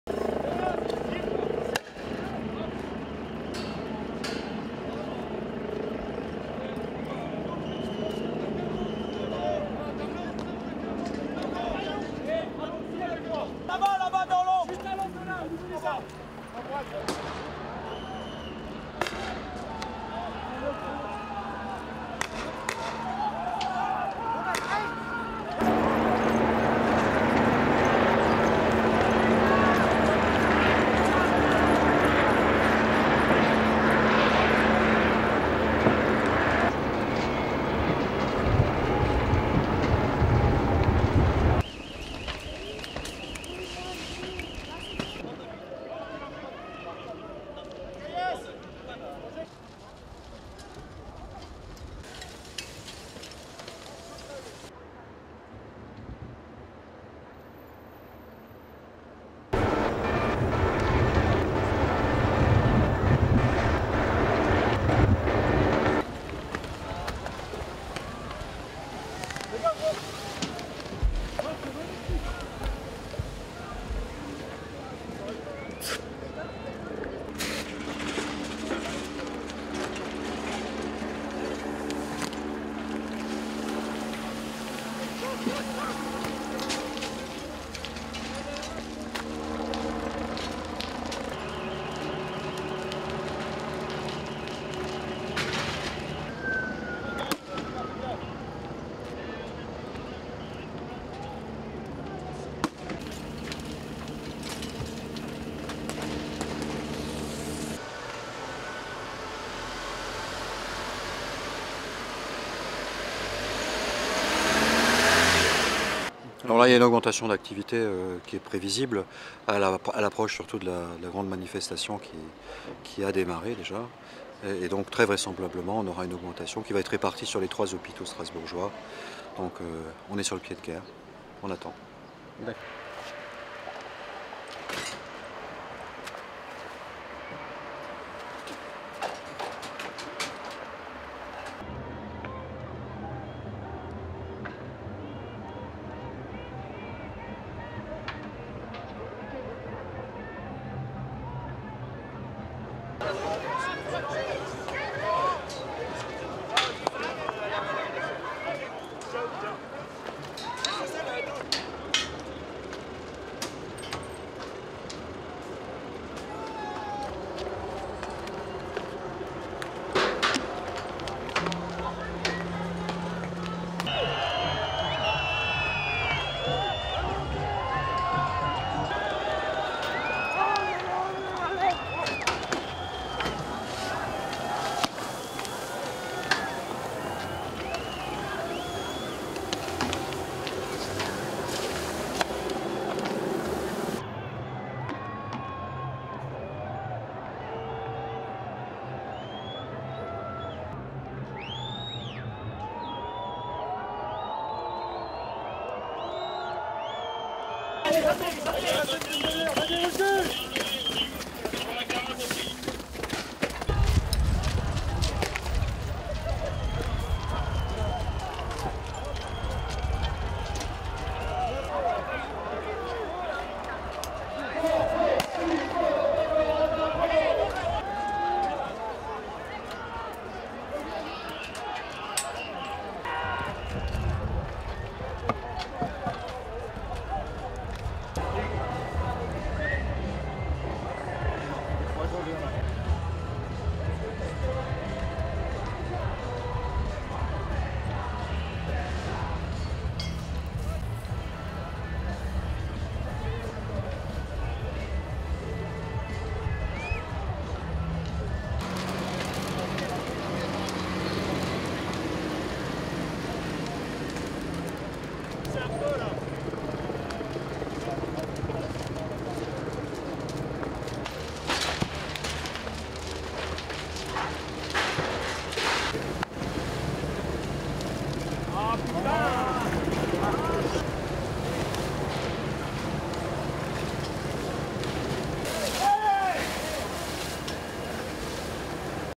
Customs house on fire, smoke over bridge, clashes, injured, demos
1. Zoom in of riot police with shields and one carrying stun gun, advancing towards protesters
2. Reverse shot of police advancing towards protesters
3. Protesters dispersing in front of hotel, teargas smoke
4. Wide pan of customs building on fire, thick smoke rising, to protesters running and police advancing, hotel on fire in background
5. Mid of hotel on fire, flames and black smoke
6. Wide of customs building on fire
7. Various of protesters outside customs building
8. Closeup customs building fire
9. Helicopter flying overhead
10. Wide of bridge, smoke billowing
11. Various of protesters near bridge amidst tear gas
12. Bus station building on fire
13. Mid of riot police police with stunguns
14. Bus stop building on fire, pan to street
15. Ambulance arriving at hospital
16. SOUNDBITE (French) Jacques Kopferschmitt, Head of emergency unit at Nouvel Civic Hospital:
"There's an increase in the activity (of the hospital) closer to the big demonstration that has started already. And so very likely we will have an augmentation, all throughout the three hospitals of Strasbourg. We are on a war footing. We are staying put."
17. Injured person being loaded into ambulance
18. Pan from police on bridge to protesters throwing objects
19. Protesters throwing objects at police line on bridge
20. Close of police line
21. Protesters throwing objects, smoke from tear gas
22. Top shot of scattering protesters near bridge
23. Wide of police barricade on bridge, police firing water canons
24. Wide side shot of police firing protesters with water canons
25. Wide pan of police firing water at scattering protesters
26. Riot police running up steps
27. Zoom in on injured police officer on ground
28. Wide of nearby protesters amidst tear gas
29. Protesters walking, makeshift barricade in background
30. Reverse shot of riot police
31. Various of protesters, makeshift barricade
32. Fire engine arriving
33. Close of customs building on fire
34. Wide of customs building on fire
35. Various of firefighters extinguishing fire
STORYLINE
Black-clad protesters attacked police and set a hotel and a customs station ablaze on Saturday, as the 60th anniversary NATO summit was underway on the French-German border.
The attacks took place near a bridge that served hours earlier as the backdrop for a show of unity by NATO leaders.
Other protesters stormed a nearby hotel, setting fires and pilfering alcohol from the hotel bar.
There were intermittent scuffles between police and demonstrators as they gradually made their way back from the Europe Bridge and tried to enter the city centre.
Around 2:45 p.m. local time (1245 GMT), the protesters, throwing rocks, tried to storm a massive police blockade at the Pont d'Anvers bridge, and were driven back by water cannon, tear gas, flash bombs and rubber bullets.
Across the canal, nearly one-thousand people gathered to watch the fracas.
Some of the protesters were hurt, but none of the injuries appeared to be serious.
Elsewhere, stacks of old tyres were also set ablaze, unleashing thick plumes of black smoke that could be seen from across the river.
Some of the protesters said they want an end to war and call NATO a tool of Western imperialism.
Others simply appeared bent on causing chaos.
Saturday's protest began calmly but began turning violent around noon (1000 GMT) at the same Pont d'Anvers bridge.
Protesters have been frustrated by large police presence in both cities.
Some 15-thousand German police and nine-thousand French police are on call for the summit.